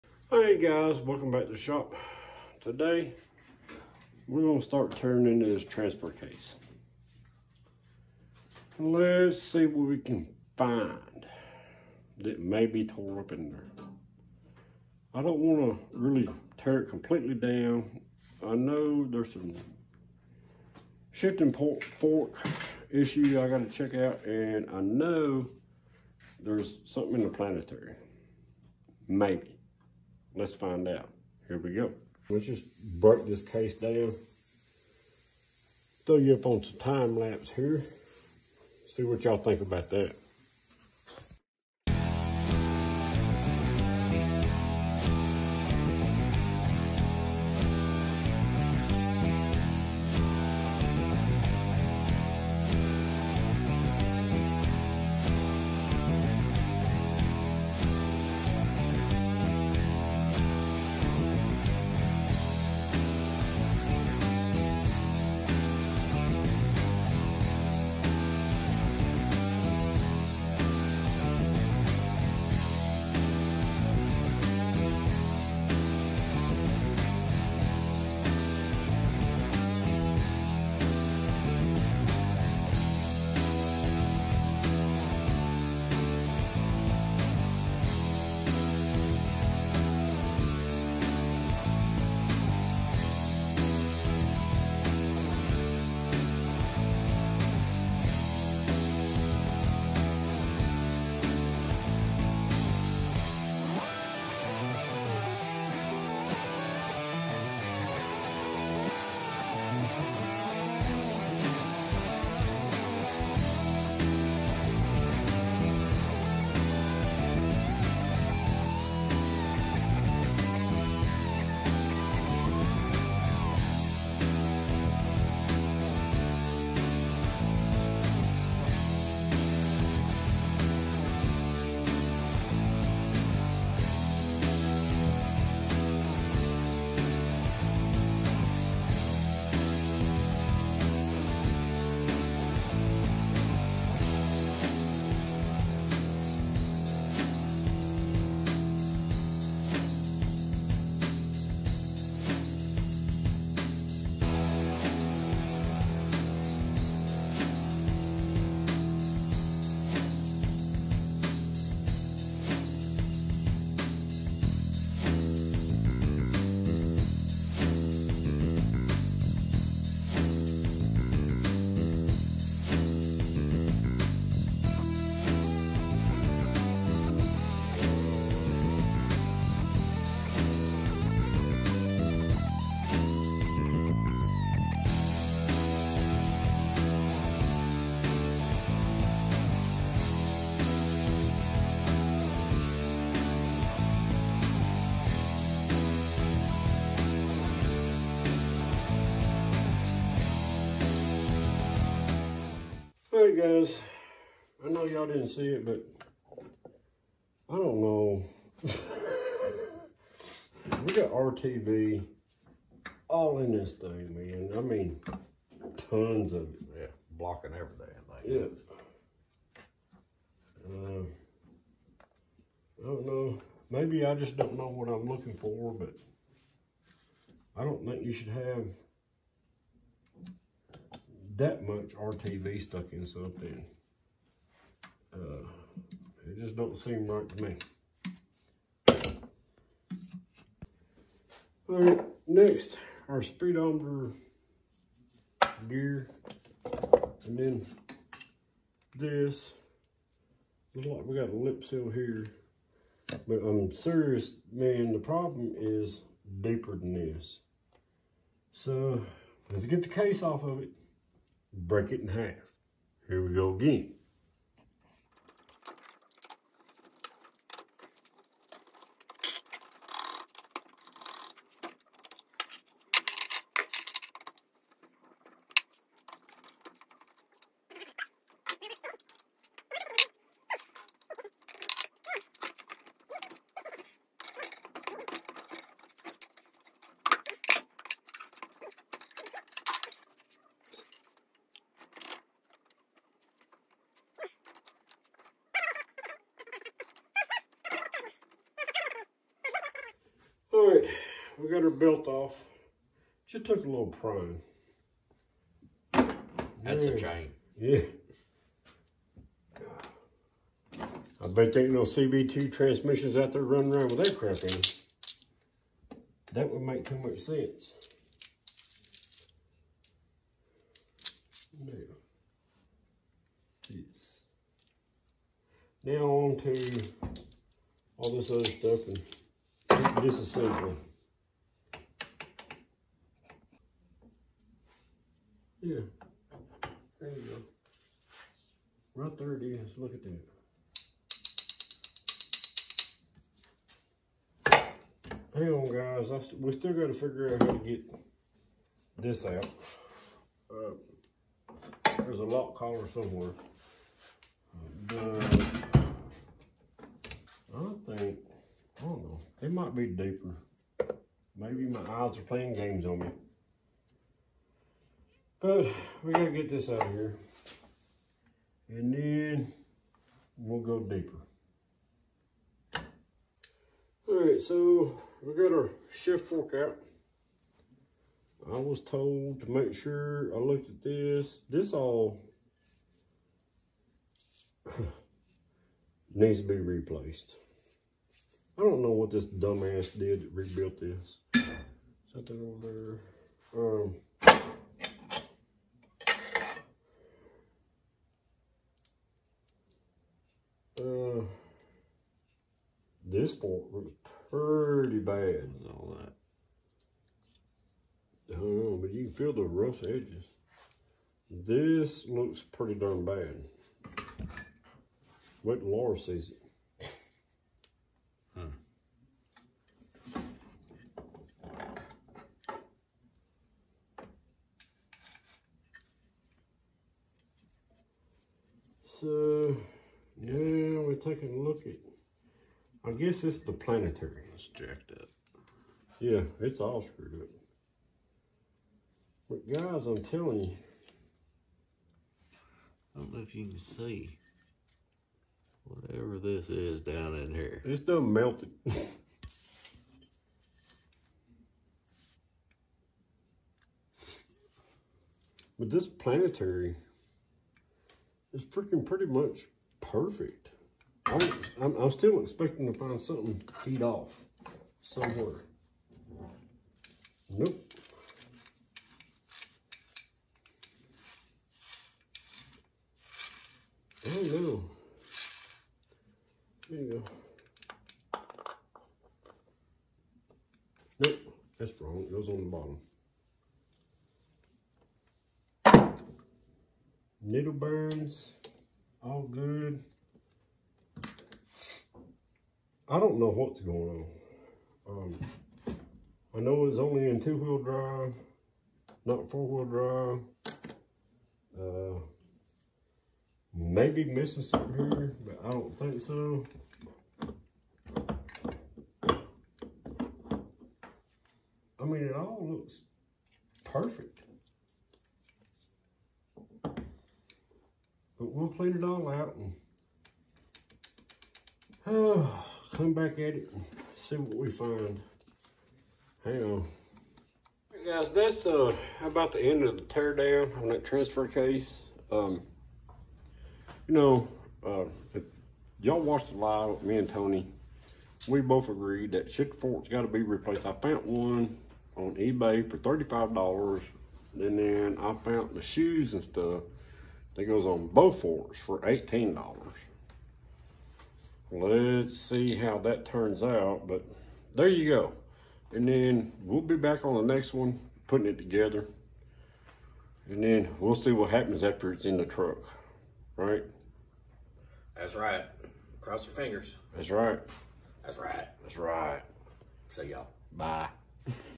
207 NP transfer case repair
207 NP transfer case slipping bad and making a bad grinding noise. Let’s see if we can find the problem.